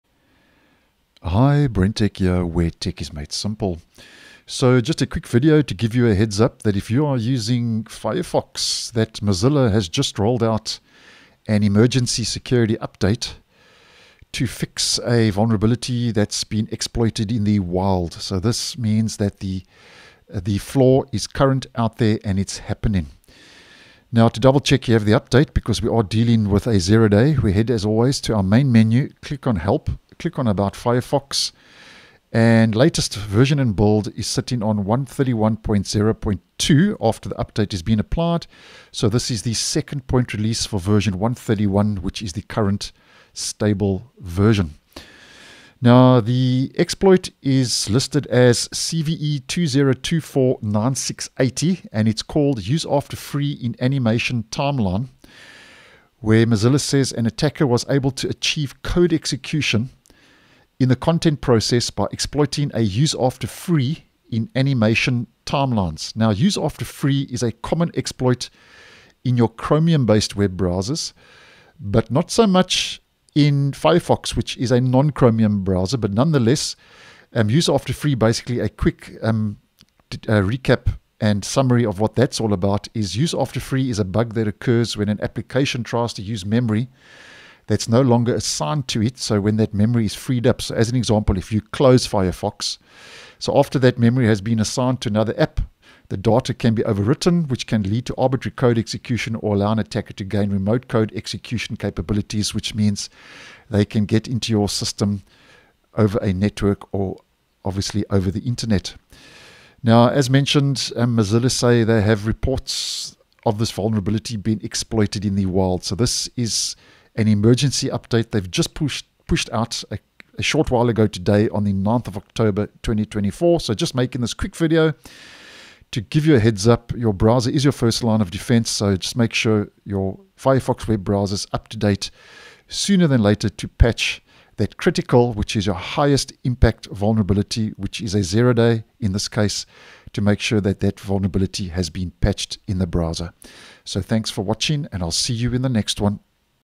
IMPORTANT Firefox 131.0.2 Emergency Security Update Patches a Critical Zero-Day!
Mozilla has released the second point release for Firefox 131, that addresses a Critical Impact security flaw exploited in the wild.